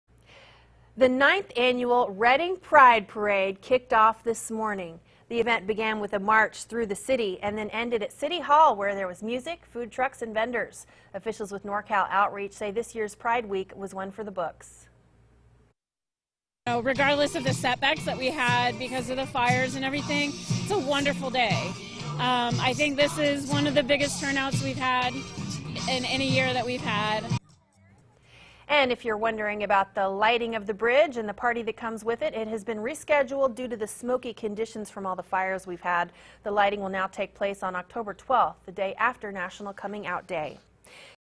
Redding Pride Paradise 2018
Here is coverage of the Redding Pride Parade for 2018. The lighting of the Sundial Bridge for Pride Week has been postponed